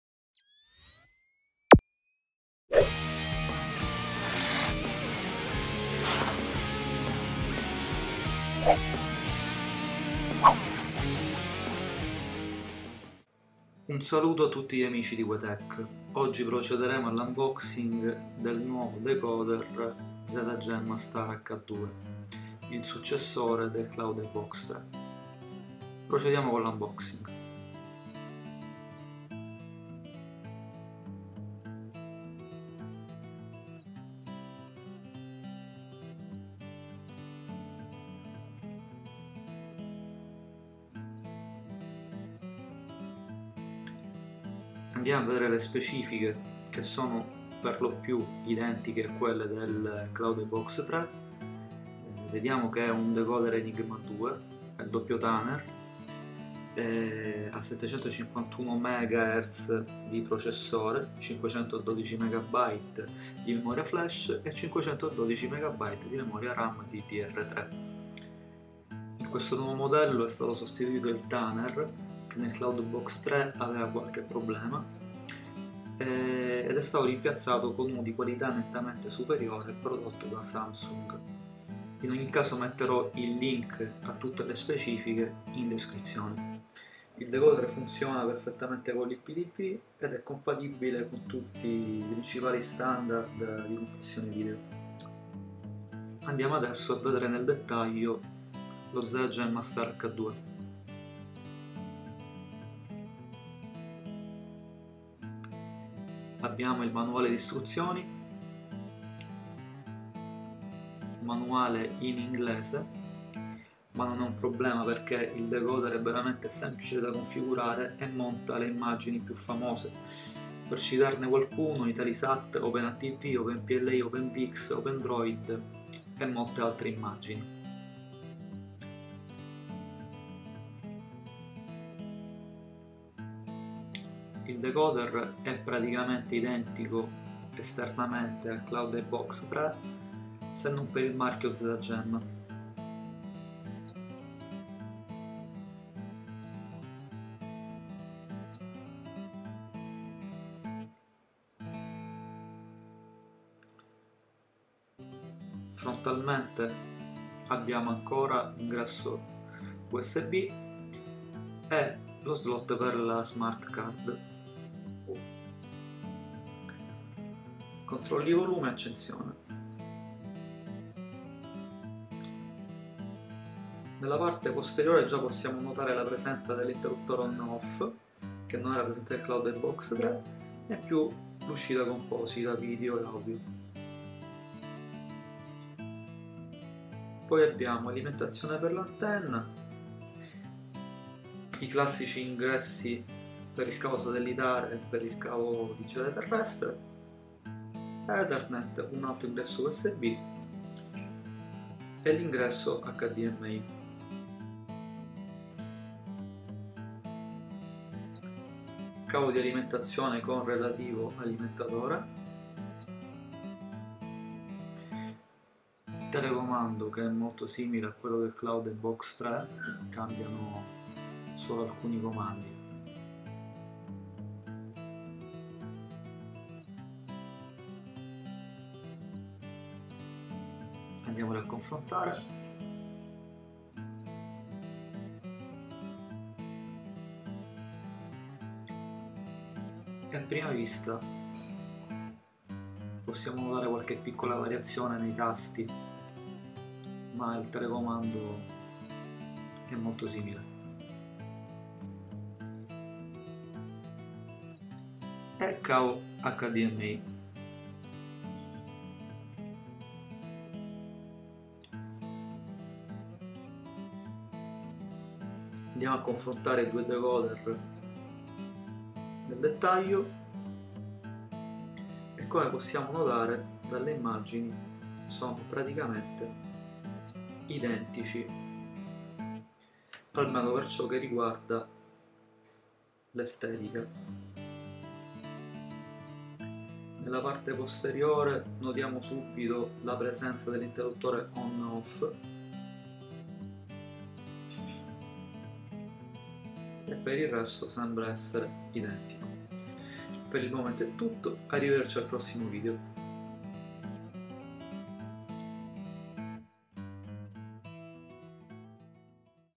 Unboxing Zgemma Star H2 e confronto con Cloud Ibox 3
Link per acquistare in offerta Zgemma H2 su Amazon

Unboxing del decoder Zgemma Star H2 e confronto con Cloud Ibox 3 se